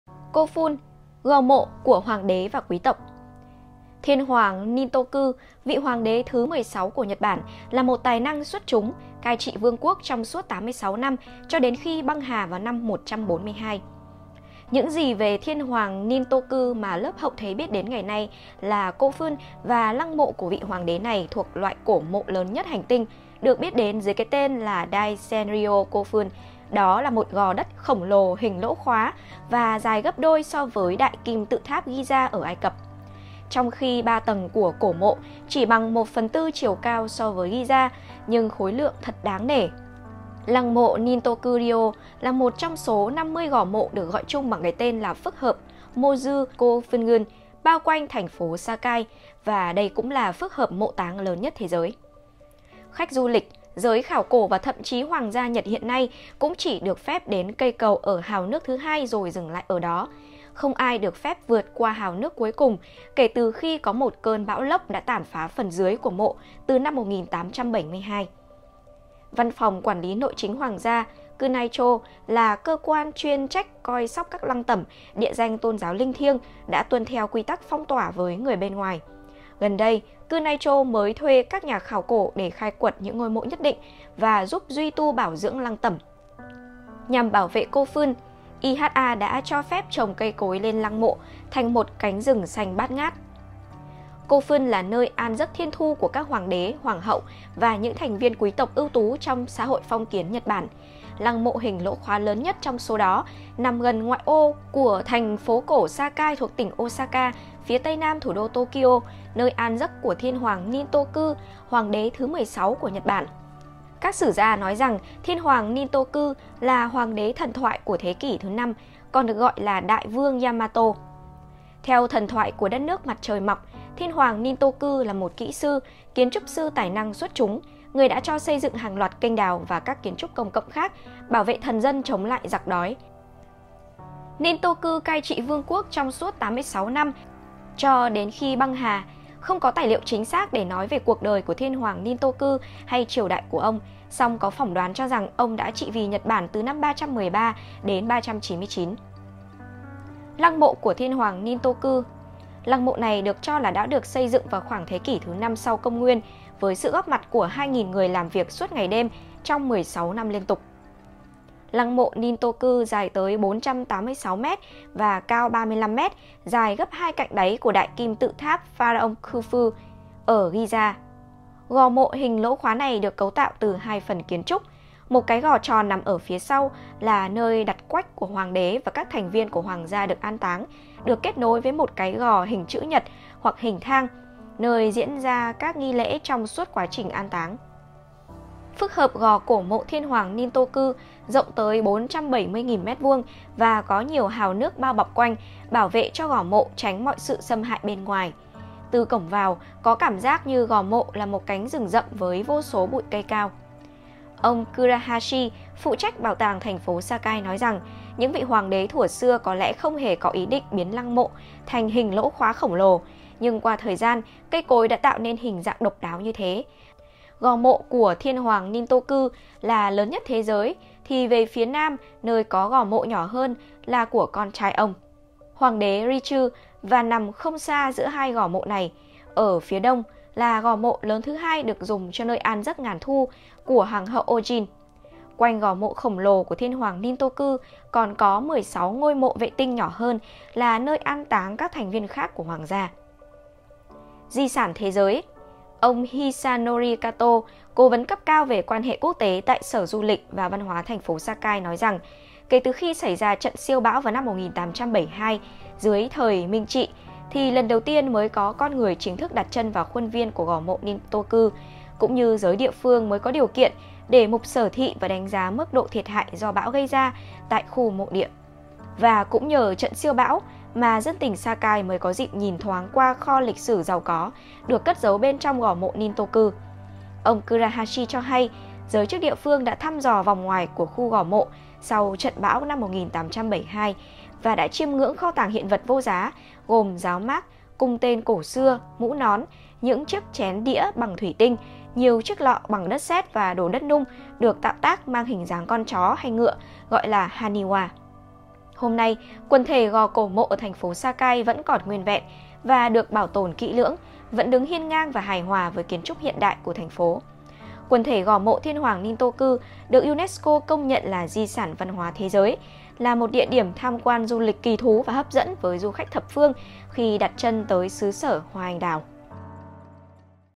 Huyền thoại ngôi mộ Thiên hoàng Nintoku
Thiên hoàng Nintoku, vị hoàng đế thứ 16 của Nhật Bản, là một tài năng xuất chúng, cai trị vương quốc trong suốt 86 năm cho đến khi băng hà vào năm 142.
Những gì về thiên hoàng Nintoku mà lớp hậu thế biết đến ngày nay là Kofun và lăng mộ của vị hoàng đế này thuộc loại cổ mộ lớn nhất hành tinh, được biết đến dưới cái tên là Daisenryo Kofun, đó là một gò đất khổng lồ hình lỗ khóa và dài gấp đôi so với Đại kim tự tháp Giza ở Ai Cập. Trong khi 3 tầng của cổ mộ chỉ bằng ¼ chiều cao so với Giza, nhưng khối lượng thật đáng nể.
Lăng mộ Nintoku-ryo là một trong số 50 gò mộ được gọi chung bằng cái tên là phức hợp “Mozu Kofungun” bao quanh thành phố Sakai, và đây cũng là phức hợp mộ táng lớn nhất thế giới.